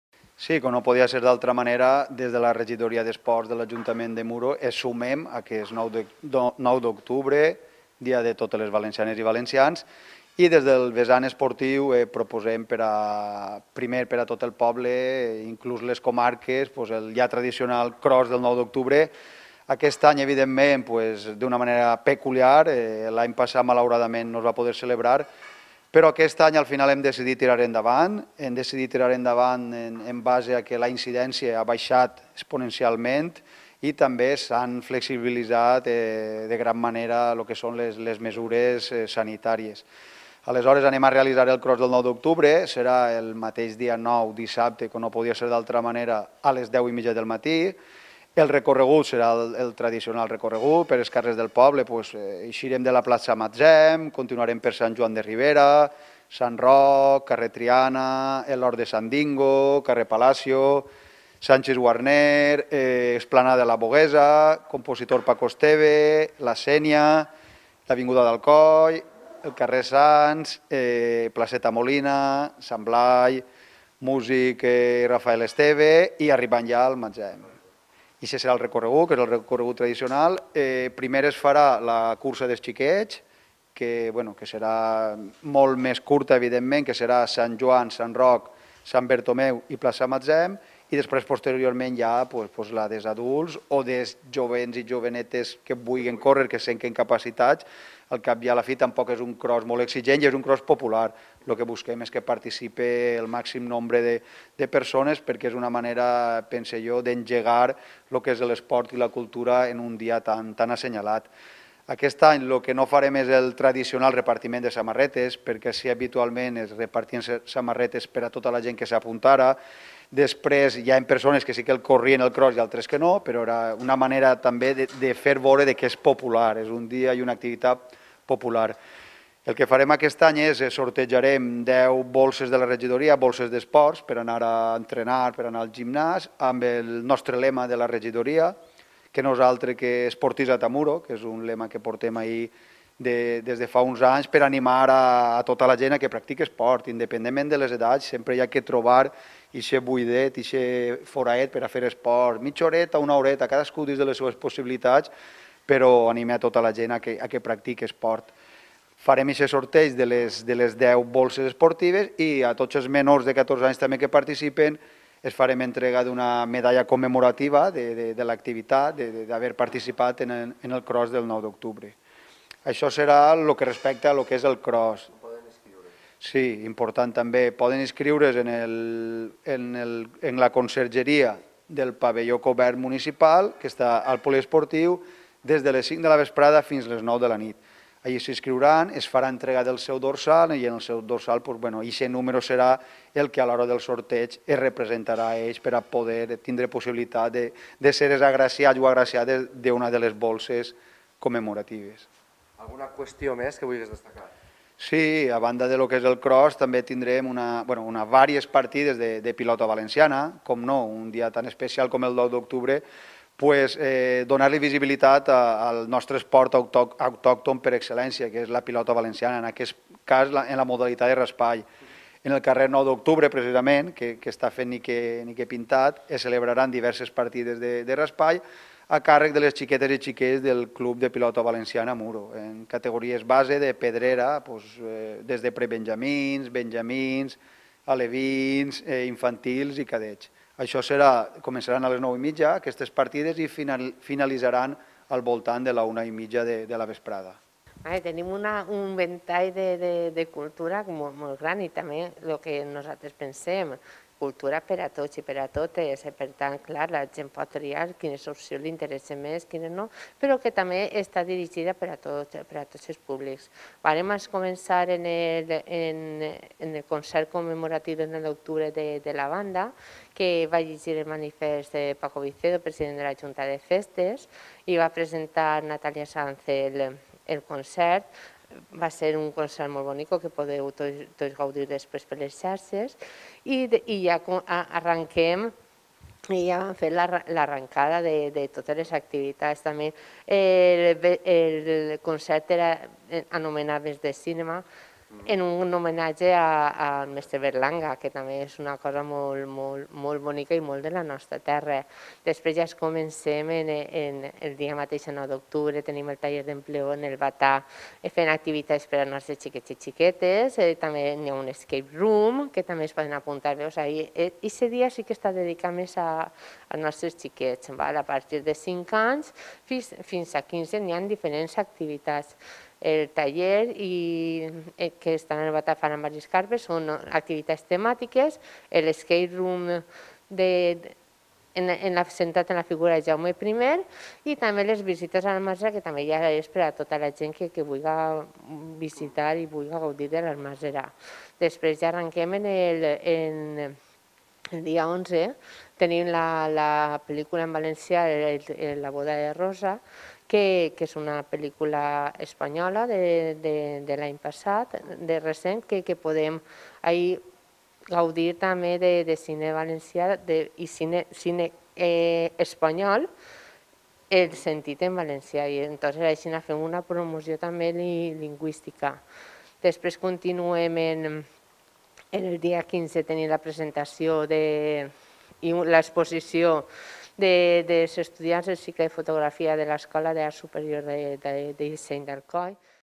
Muro: No et pots perdre la programació especial del 9 d'Octubre a Muro
Muro: No et pots perdre la programació especial del 9 d'Octubre a Muro amb esport, actes culturals, entre altres 🎉